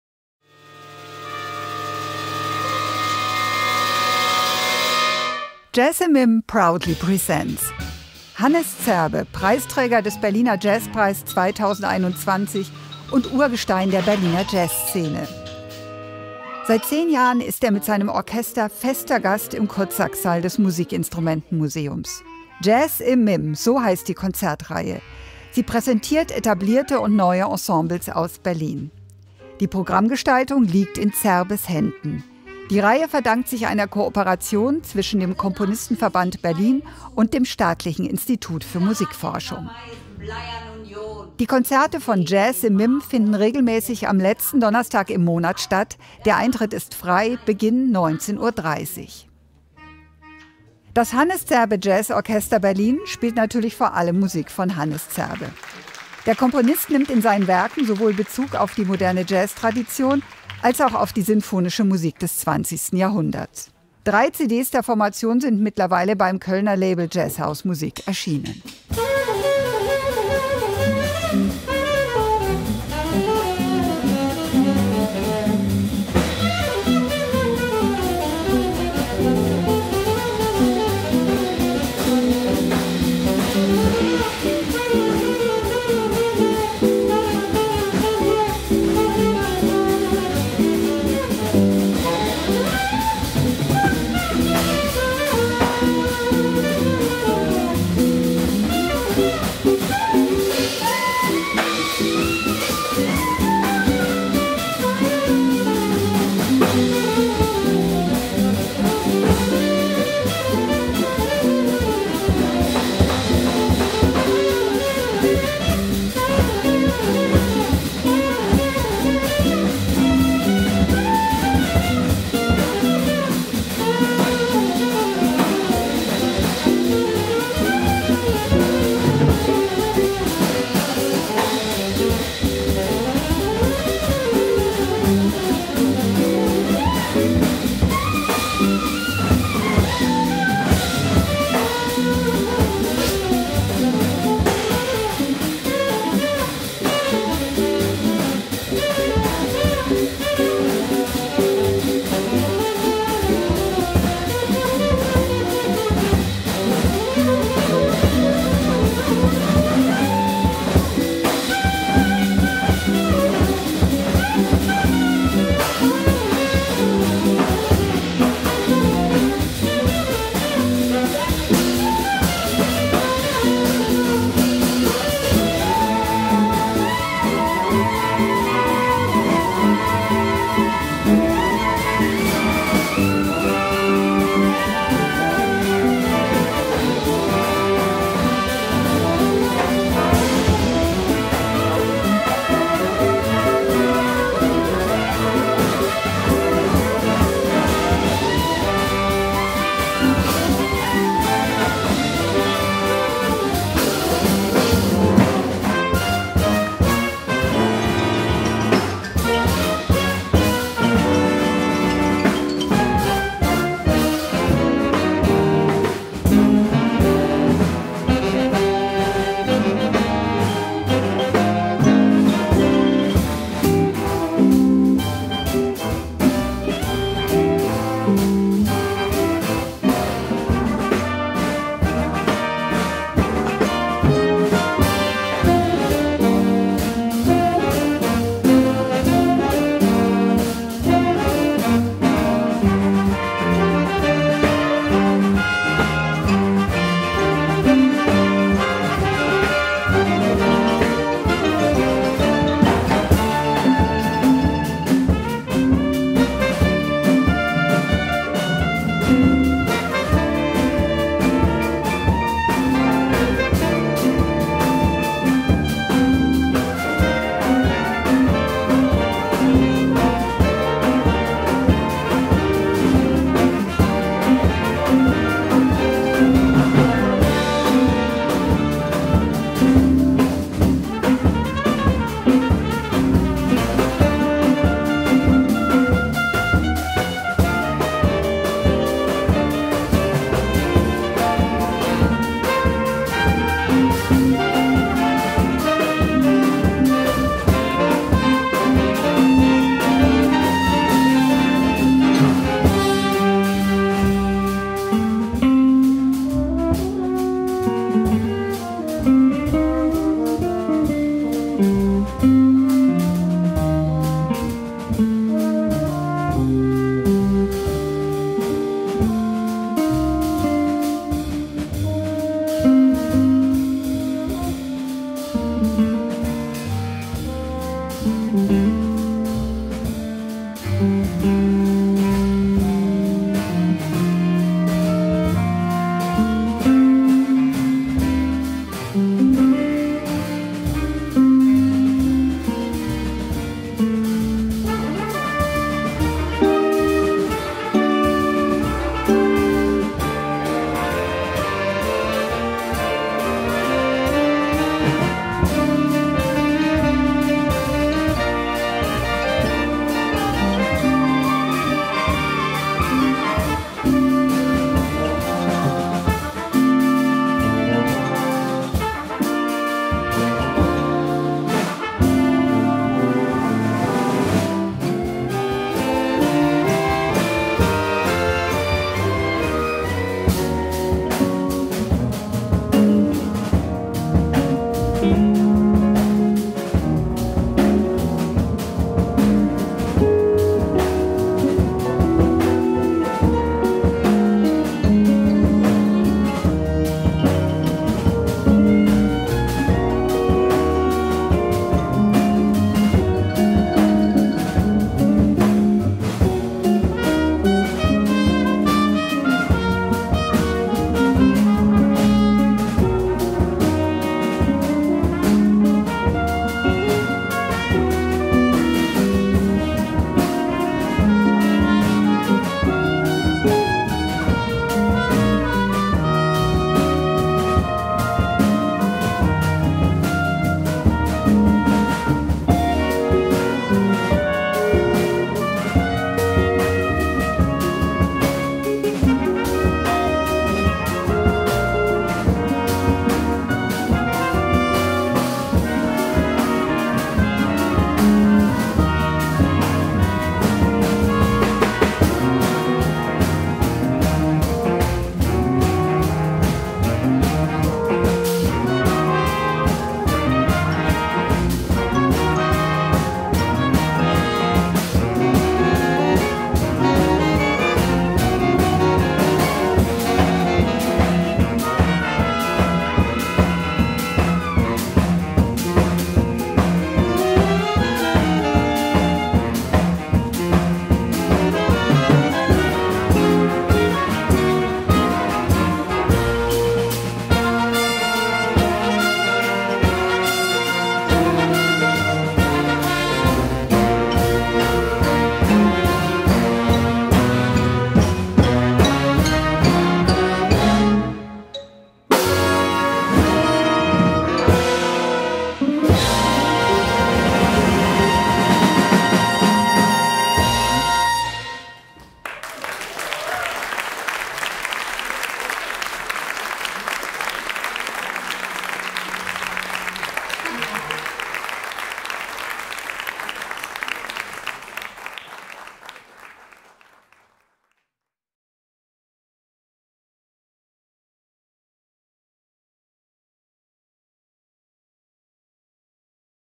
Hannes Zerbe Jazz Orchester Berlin
Berlins kulturelle Vielfalt spiegelt sich auch in der Jazz-Szene wieder. Seit über zehn Jahren bietet das Musikinstrumenten-Museum des Staatlichen Instituts für Musikforschung den vielen Berliner Ensembles im Bereich des modernen Jazz eine Spielstätte. Die Reihe »Jazz im MIM« findet in Kooperation mit dem Komponistenverband Berlin statt und wird von der Kulturverwaltung des Berliner Senats finanziell unterstützt. Die Programmgestaltung liegt in den Händen des Musikers und Komponisten Hannes Zerbe.

Das Hannes Zerbe Jazz Orchester Berlin besteht seit 2011 und hat sich beständig weiterentwickelt. Die Spielstücke stammen alle aus der Feder von Hannes Zerbe, sie nehmen Bezug sowohl auf die moderne Jazztradition
(unter anderem Gil Evans) als auch auf sinfonische Musik des 20. Jahrhunderts (wie Dmitri Schostakowitsch oder Hanns Eisler). Auch literarische Vorlagen werden, zusammen mit der Schauspielerin Heide Bartholomäus, umgesetzt. Drei CDs des Orchesters erschienen bisher beim Kölner Label JazzHausMusik.

Am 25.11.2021 spielte das HZJO im Musikinstrumenten-Museum in folgender Besetzung:

Jürgen Kupke, Klarinette
Nico Lohmann, Altsaxophon, Flöte
Silke Eberhard, Altsaxophon, Klarinette
Dirk Engelhardt, Tenorsaxophone
Alexander Beierbach, Baritonsaxophon
Florian Bergmann, Bassklarinette
Damir Bacikin, Trompete
Nikolaus Neuser, Trompete, Flügelhorn
Fritz Moshammer, Trompete
Morris Kliphuis, Horn
Sören Fischer, Posaune
Florian Juncker, Posaune
Matthew Bookert, Tuba
Taiko Saito, Xylophon, Percussion
Jörg Schippa, Gitarre
Horst Nonnenmacher, Bass
Christian Marien, Schlagzeug
Heide Bartholomäus, Stimme
Hannes Zerbe, Piano, Leitung